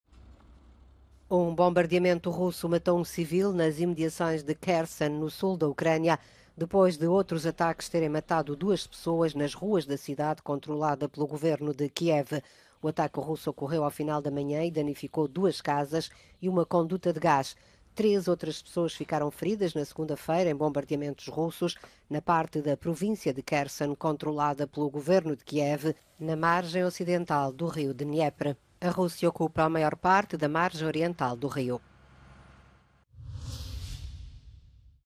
Três mortos e vários feridos, vítimas de bombardeamentos russos em Kherson
A Rússia ataca diariamente zonas habitadas na margem ocidental do último troço do rio Dnieper, que divide a província de Kherson em duas.